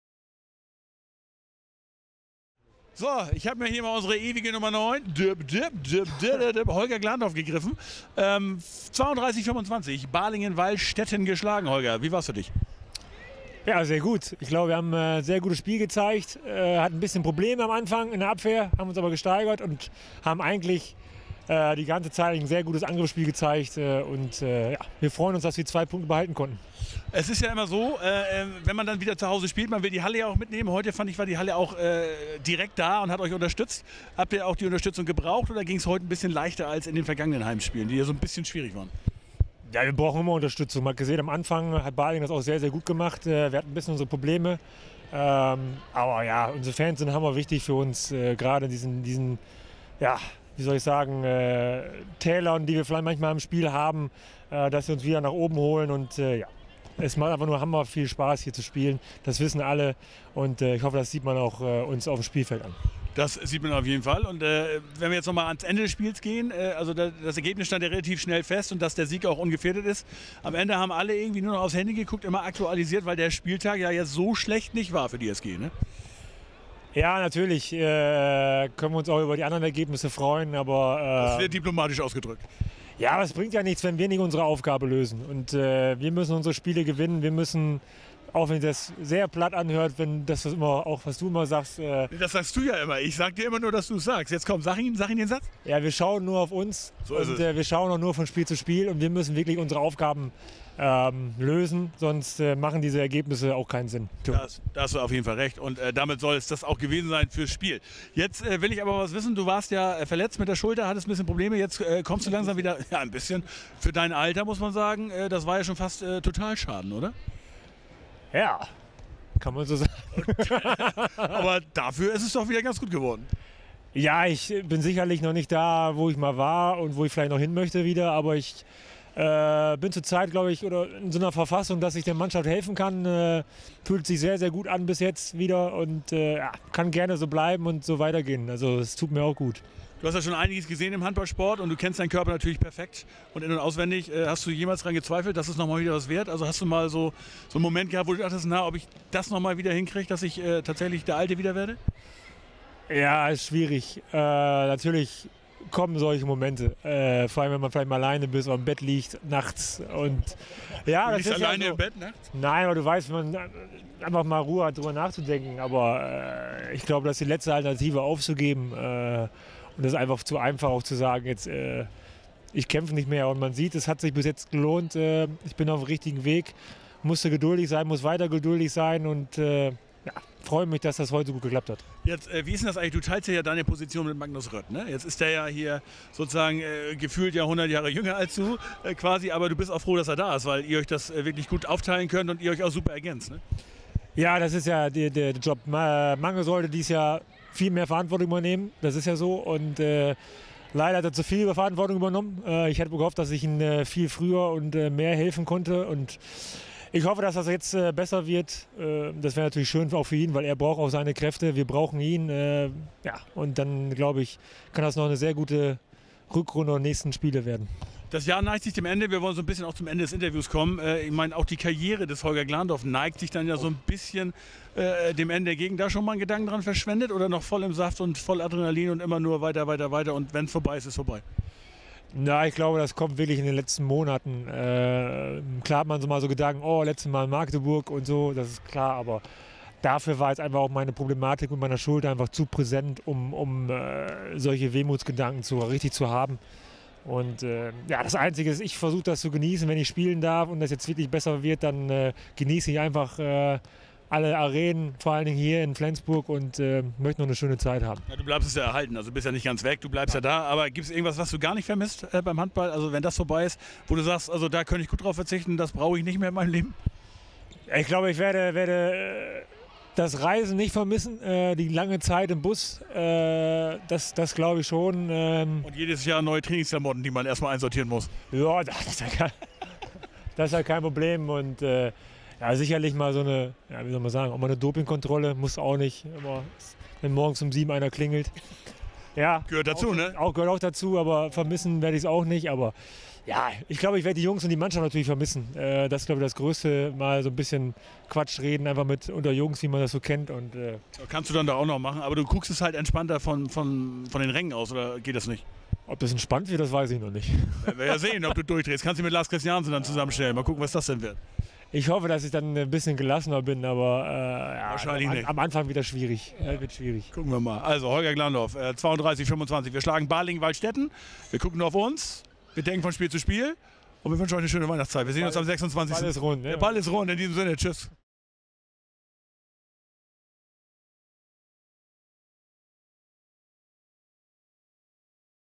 Holger Glandorf im Talk mit York
Flensburg-Handewitt siegt ohne Probleme

Der Meister aus Flensburg (26:8) gewann sein Heimspiel gegen den Aufsteiger HBW Balingen-Weilstetten überaus souverän mit 32:25 (18:13).